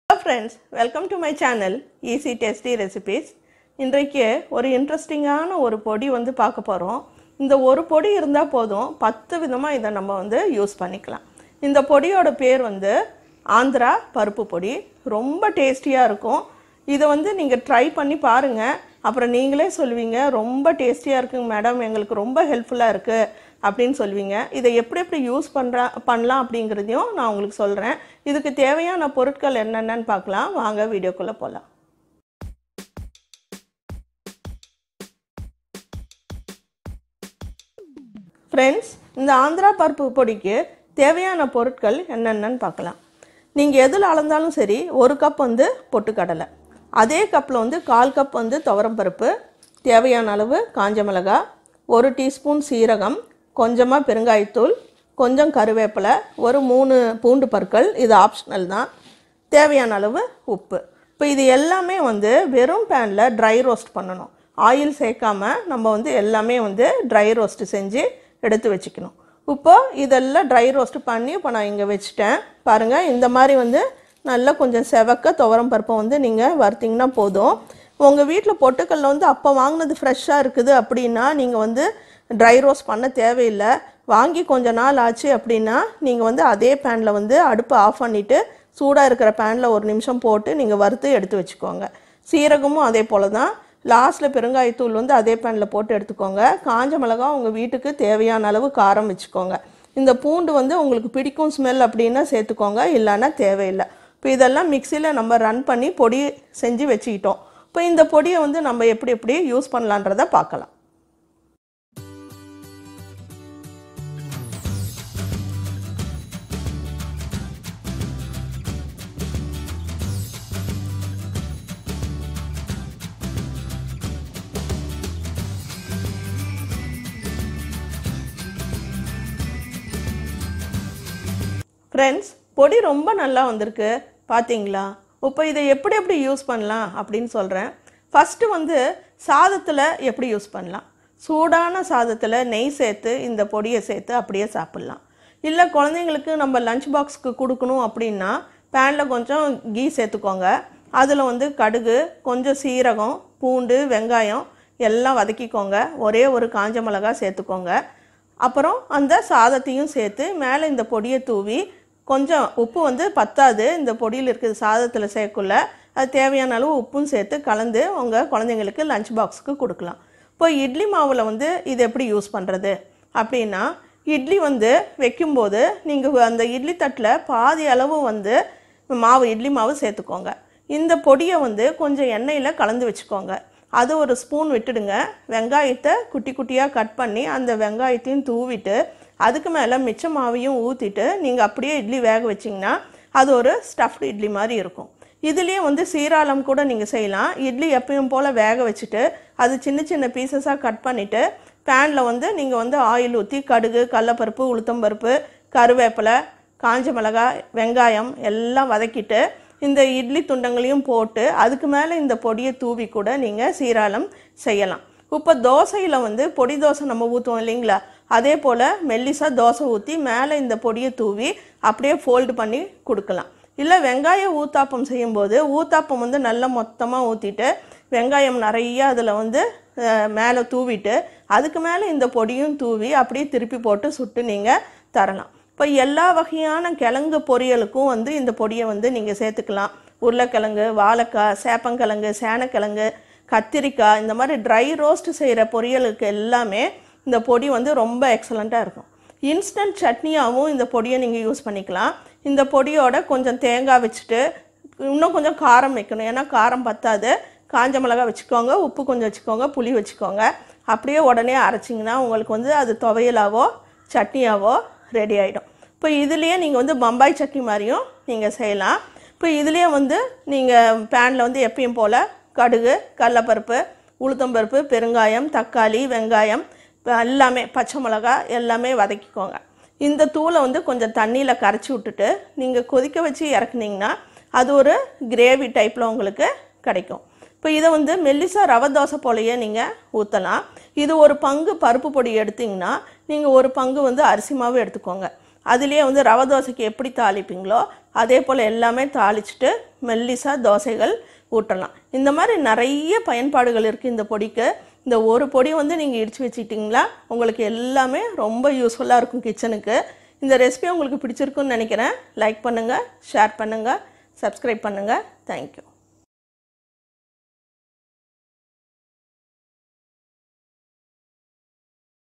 10 Uses in One Podi - Andhra Paruppu Podi
10 Uses in One Podi - Andhra Paruppu Podi
This video gives you the preparation of tasty spicy andhra podi which can be used in 10 ways.